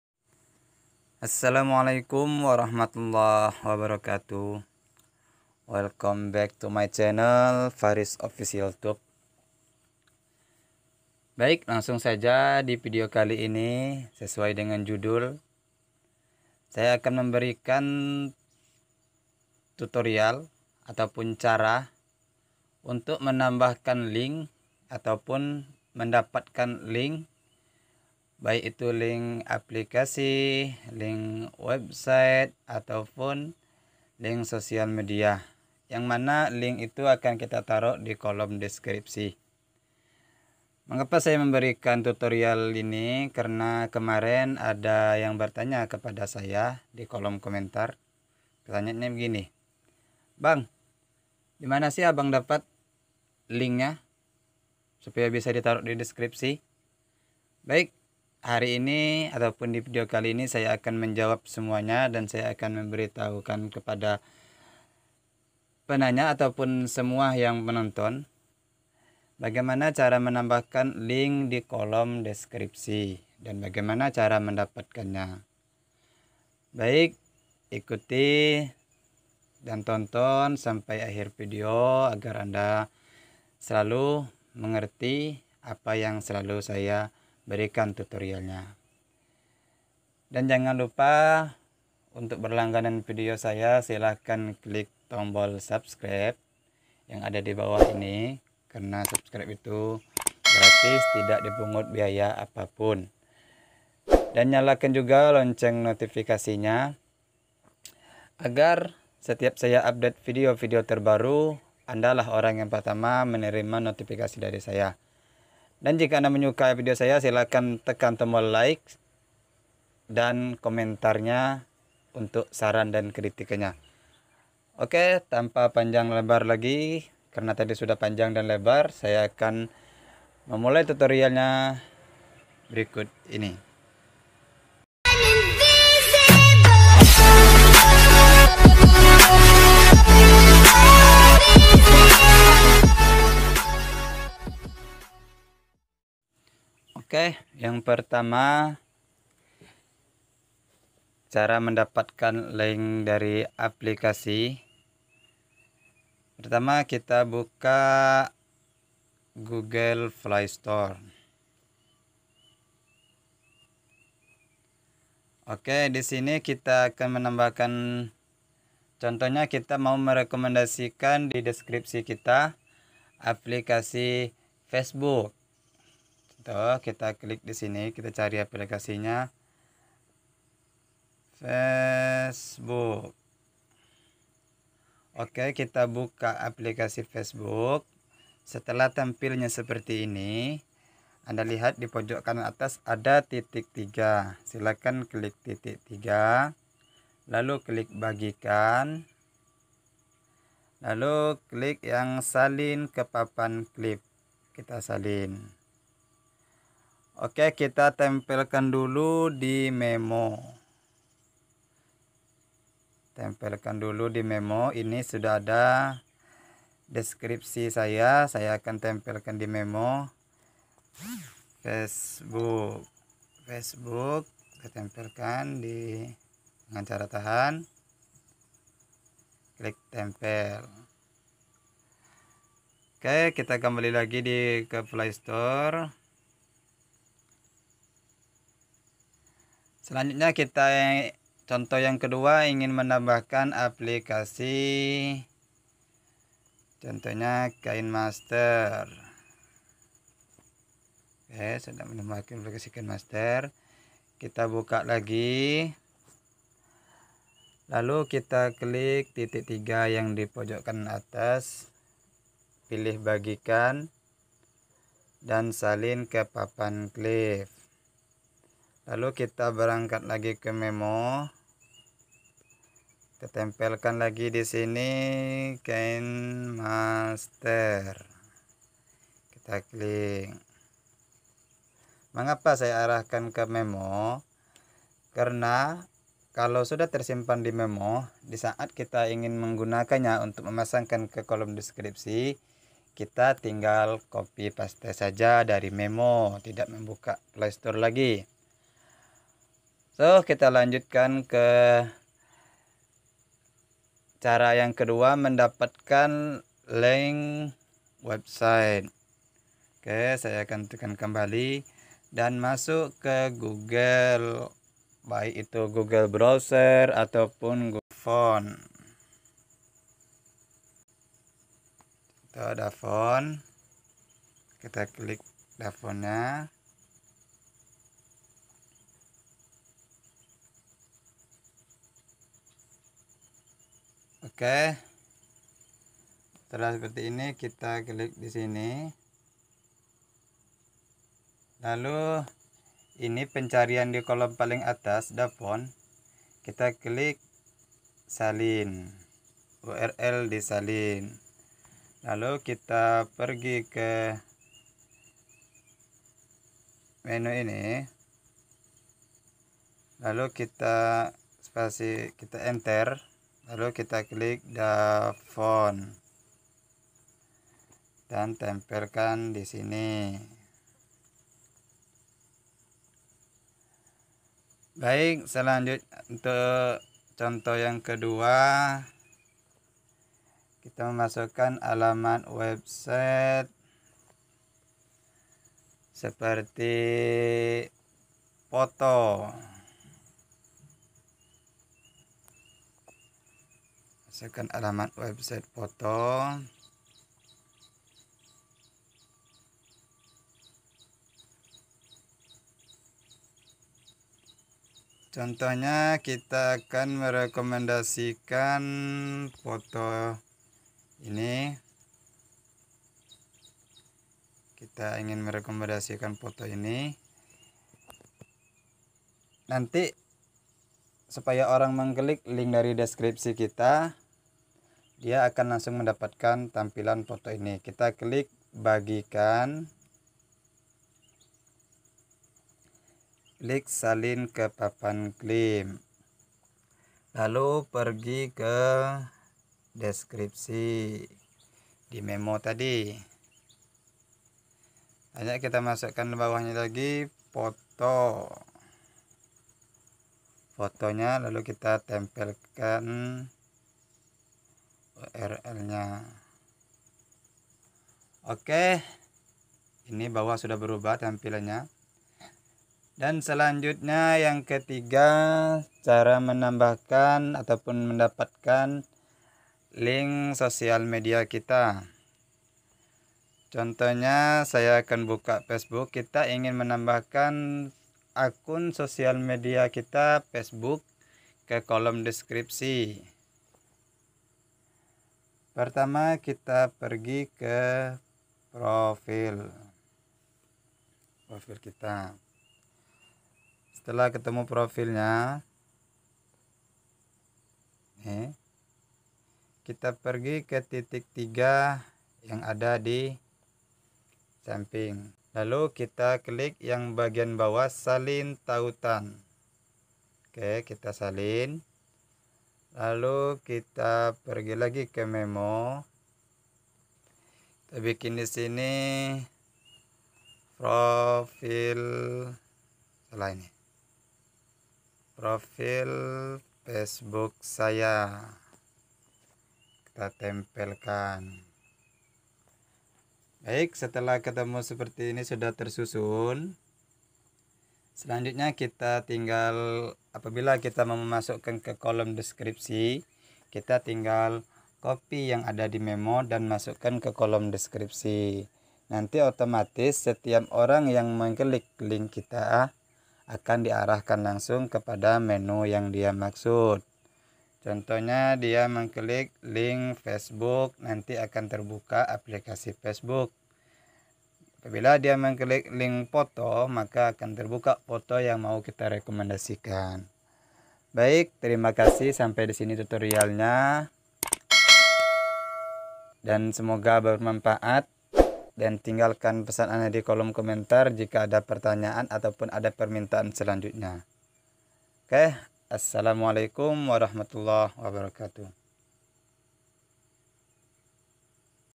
Cara mendapatkan link untuk dipasang ke deskripsi you tube dengan mudah.
Assalamualaikum. Wr.wb
Selamat datang kembali di channel Faris official tube.

Cara menambahkan link
1. Link aplikasi
2. Link website
3. Link sosmed

Aplikasi untuk edit video saya menggunakan Kine master

Dan thumbnail pakai aplikasi PIXELLAB

Terimah kasih...